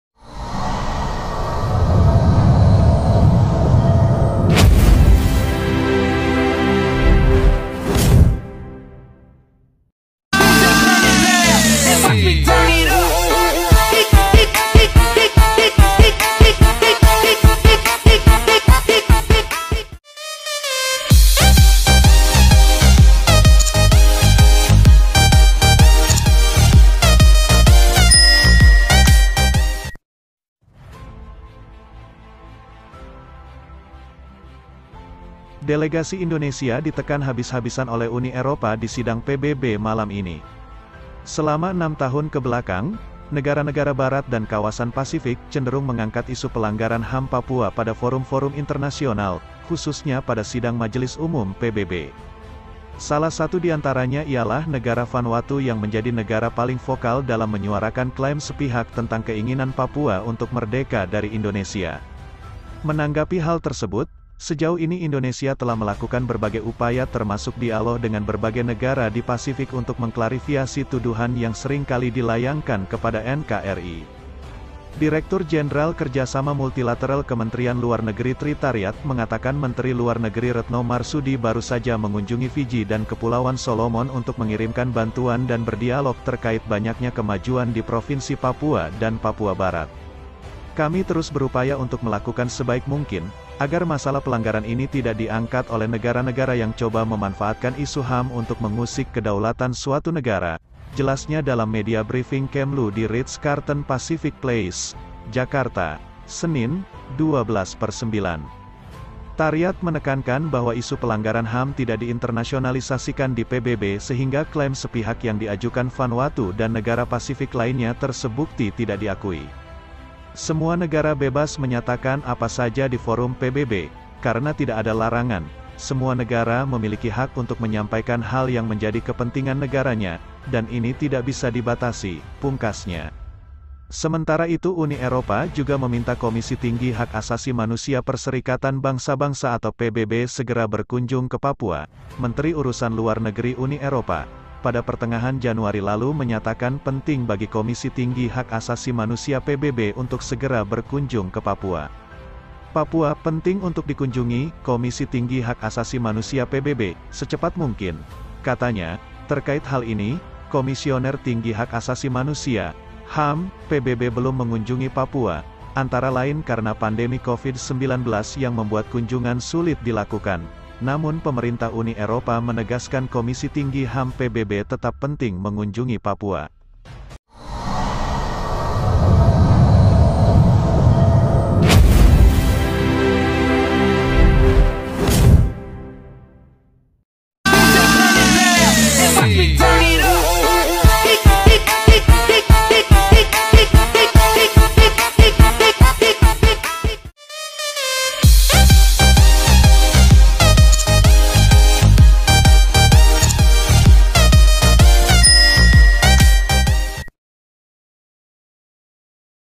DELEGASI INDONESIA DITEKAN HABIS-HABISAN UNI EROPA DI SIDANG Umum PBB ke 77
DELEGASI INDONESIA DITEKAN HABIS-HABISAN UNI EROPA DI SIDANG Umum PBB ke 77. Pelanggaran HAM Berat diwest Papua Sidang umum PBB tahun 2022.

Terima kasih berkunjung Serta SupportNya🙏